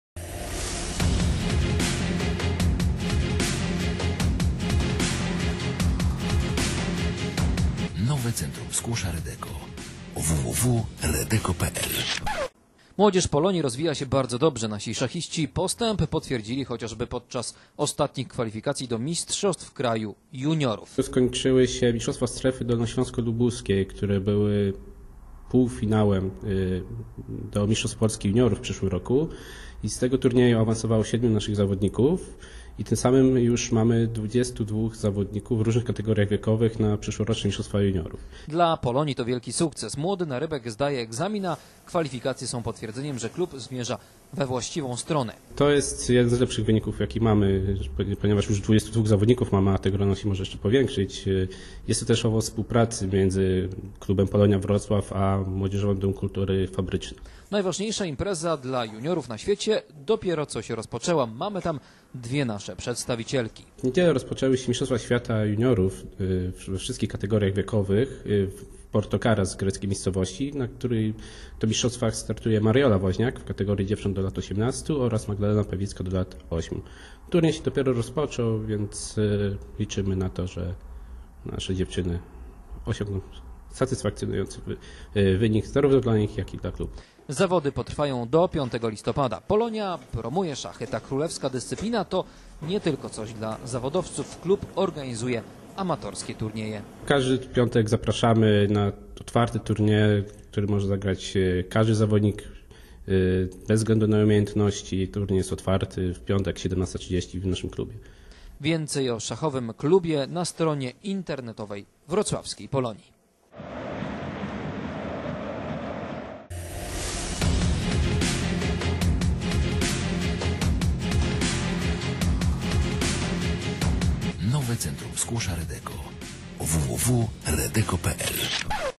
Dzieje się w szachowej Polonii
Sporo dzieje się w szachowej Polonii Wrocław. Za nami IX Kongres dr Jana Zukertorta. Za nami też rywalizacja będąca kwalifikacją do mistrzostw Polski juniorów. To nie koniec zmagań. Reprezentacji klubu walczą też w Mistrzostwach Świata Juniorów w Porto Carras.